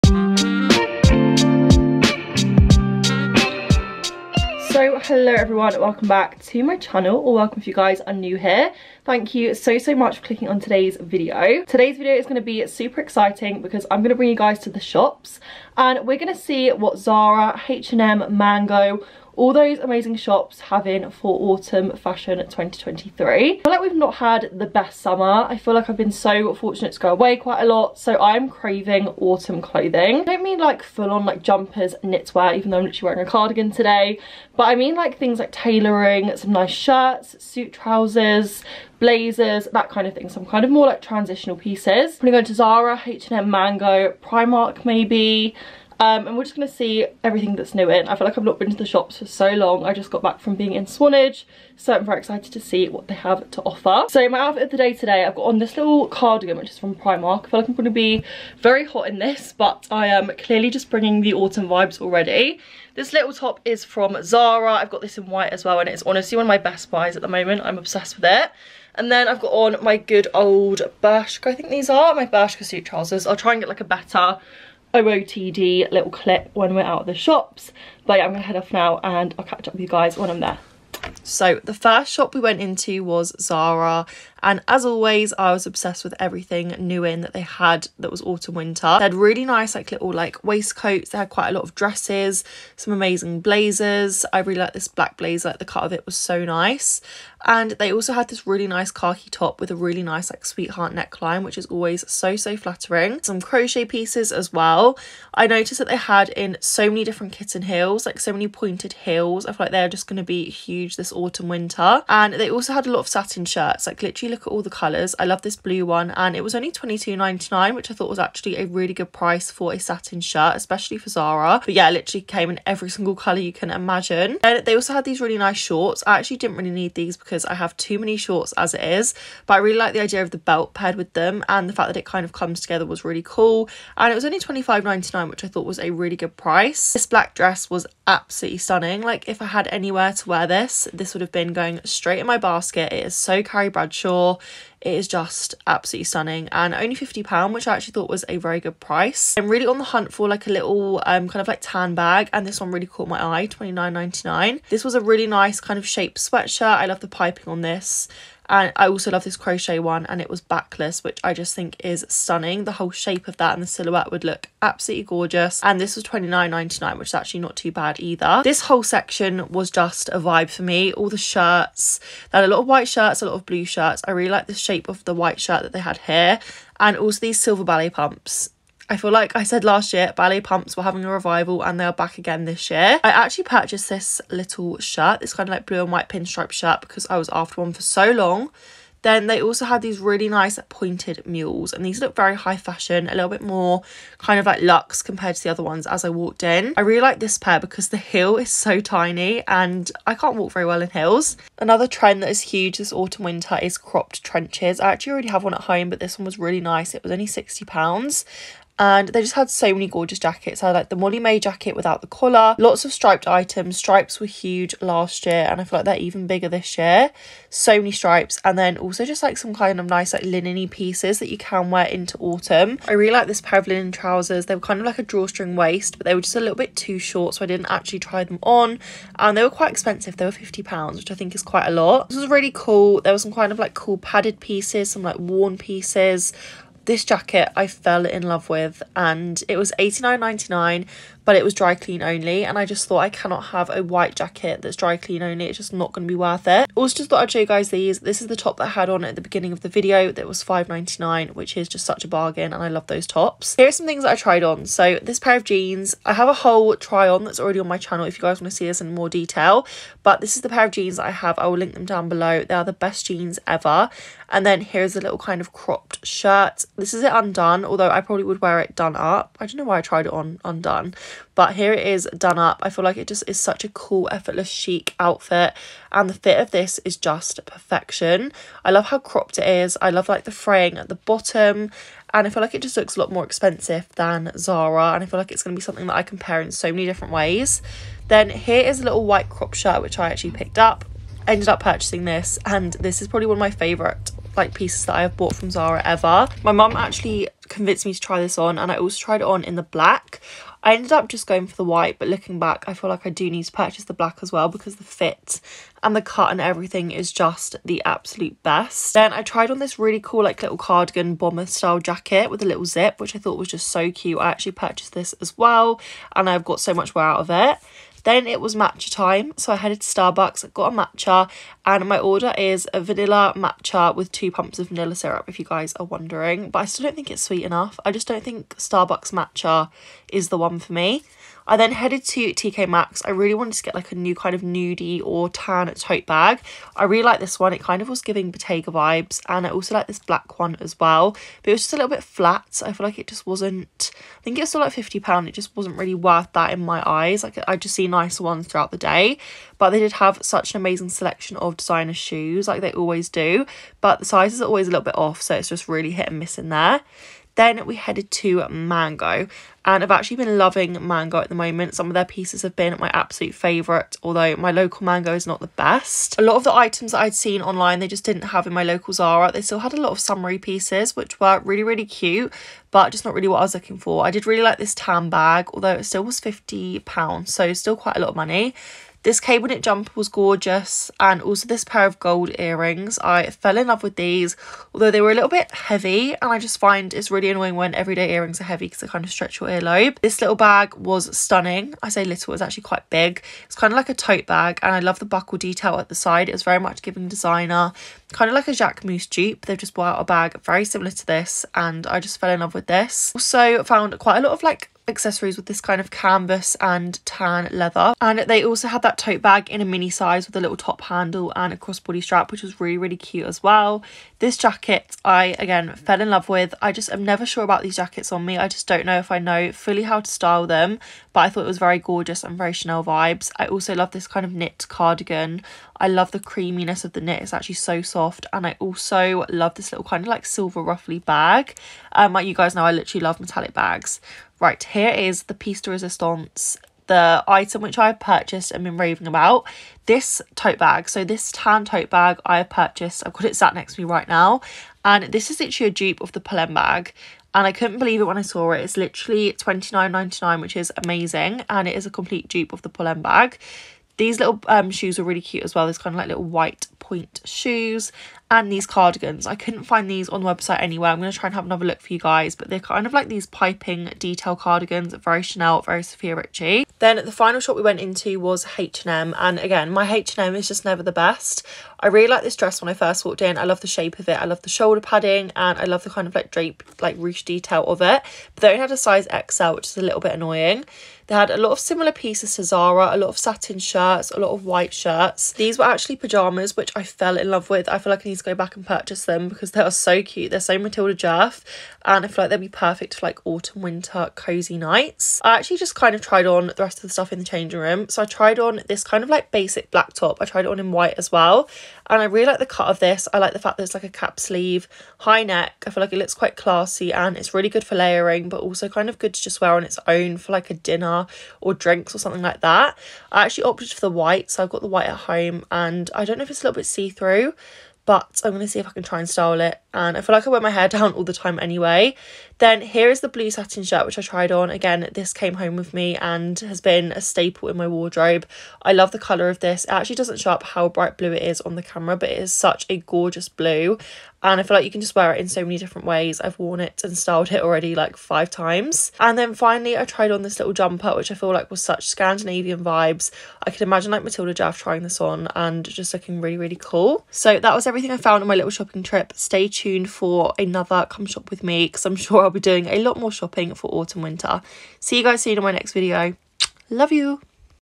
COME SHOP WITH ME VLOG! NEW IN ZARA, MANGO & H&M - AUTUMN 2023
Hi guys!

Todays video is a ‘come shop with me’ where I show you all things new in! I hope you enjoy🫶🏼